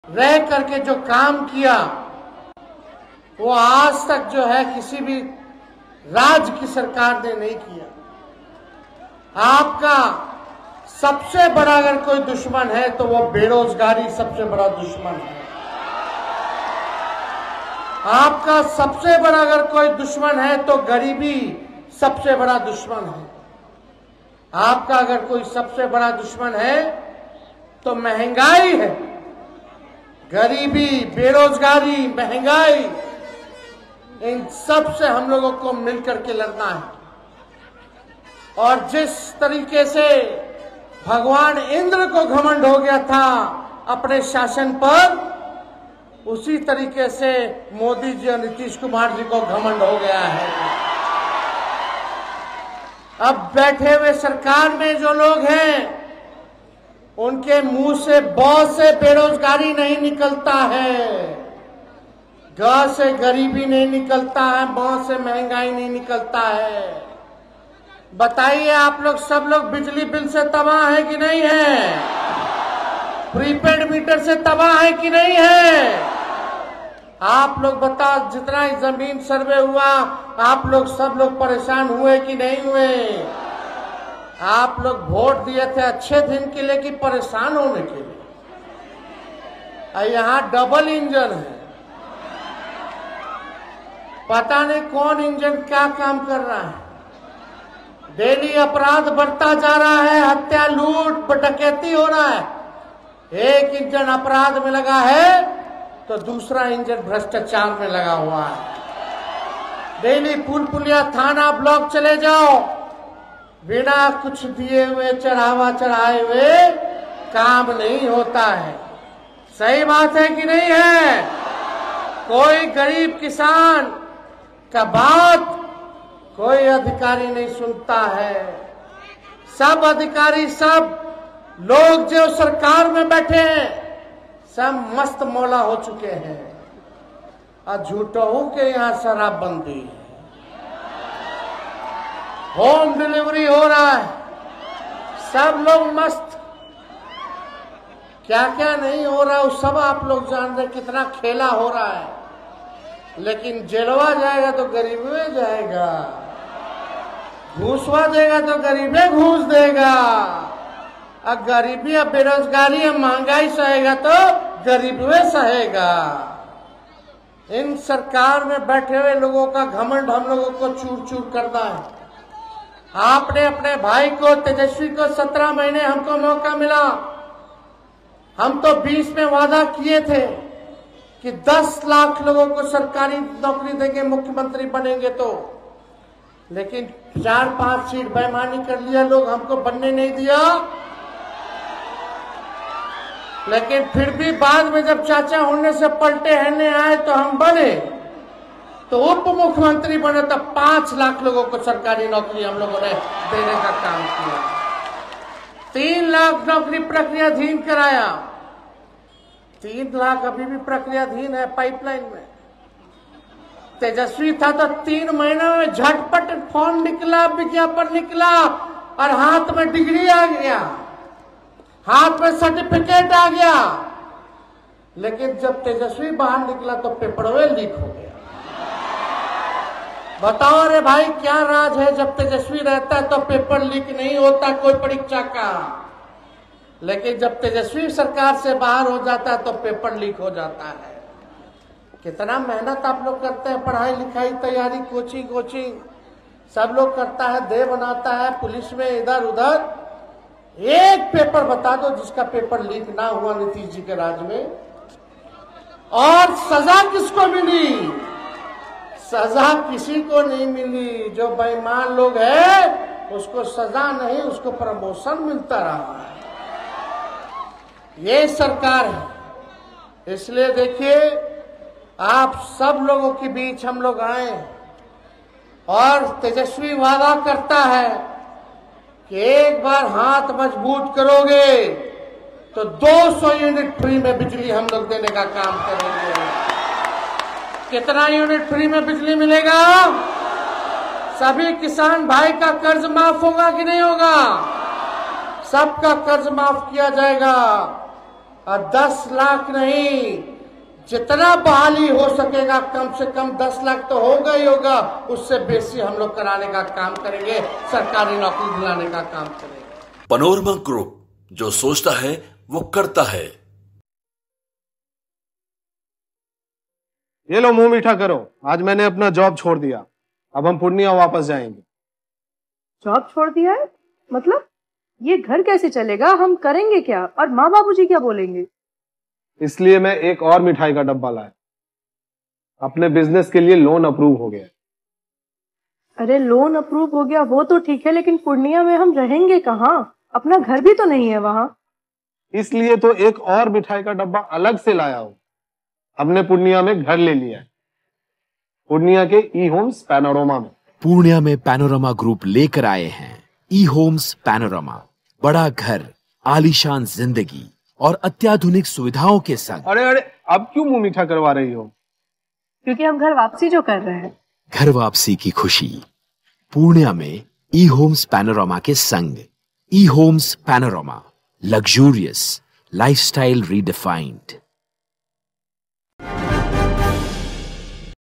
Tejashwi Yadav ने मंच से भरी हुंकार, Nitish पर जमकर साधा निशाना,जमीन सर्वे पर बहुत कुछ बोले,सुनिए
3.Bihar Government Jobs : खुशखबरी! Bihar में जल्द आने वाली है इन दो विभागों में बम्पर भर्ती